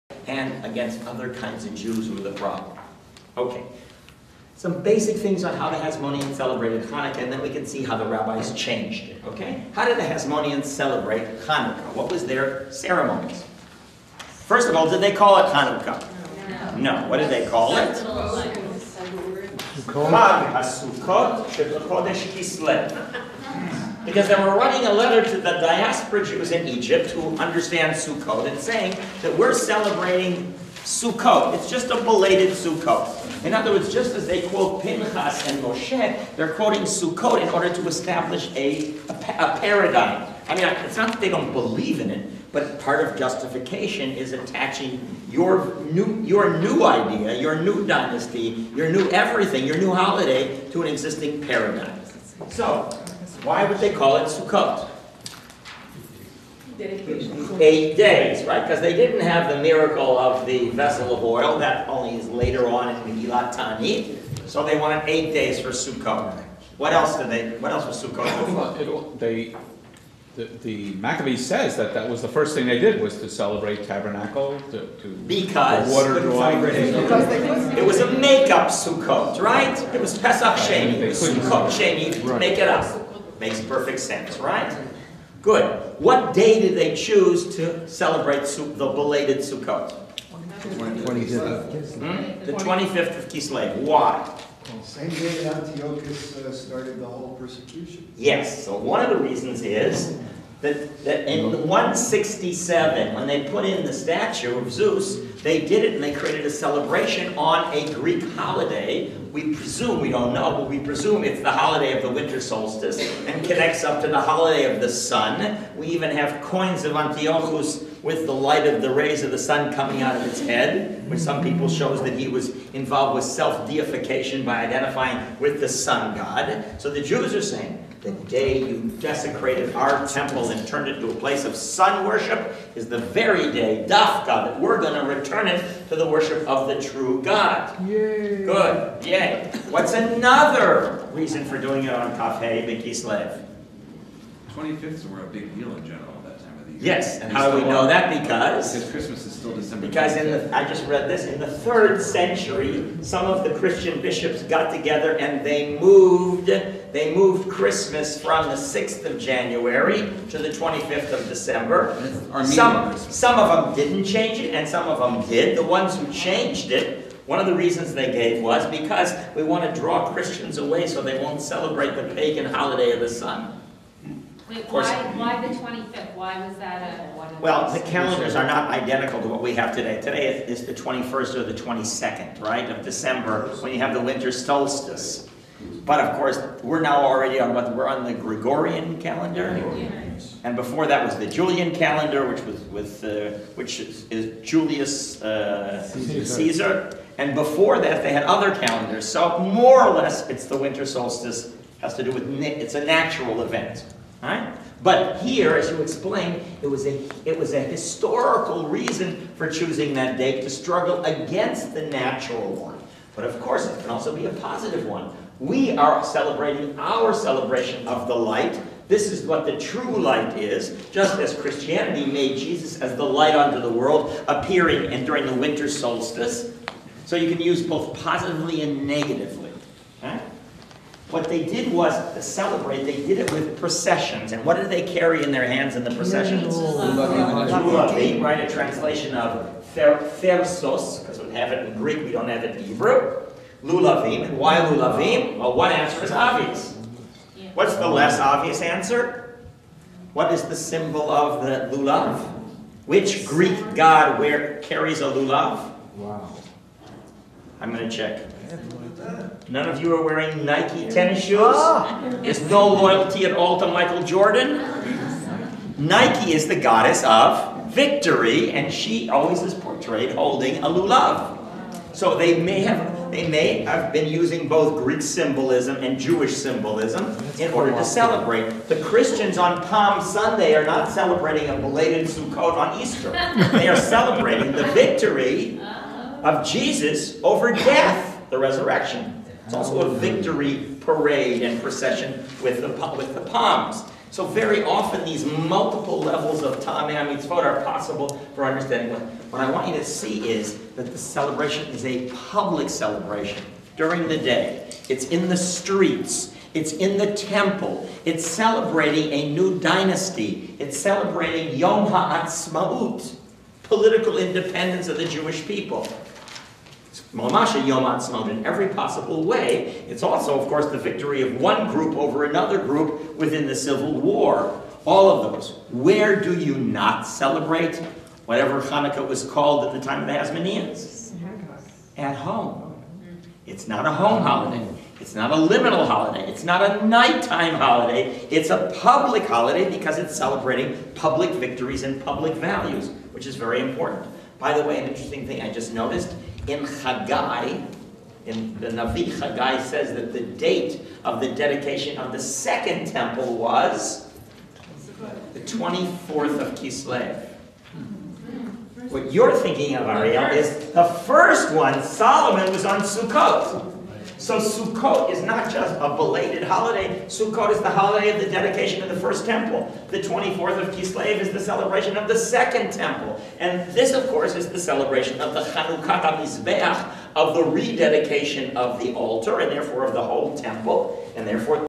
Noam Zion: The Reinvention of Hanukkah in the 20th Century, Pt. 3
The Reinvention of Hanukkah in the 20th Century: A Jewish Cultural Civil War Between Zionists, Liberal American Judaism and Habad - Who Are the Children of Light and Who of Darkness?
with Noam Zion

Noam Zion is a fellow at the Shalom Hartman Institute and holds a Master of Arts in philosophy from Columbia University. His numerous publications include: A Different Night: The Family Participation Haggadah, A Different Light: The Big Book of Hanukkah, A Day Apart: Shabbat at Home, Sipurei Reshit, a Hebrew anthology on contemporary readings of Genesis that he published with his daughter, and Halaila Hazeh and A Night to Remember, haggadot that he published with his son.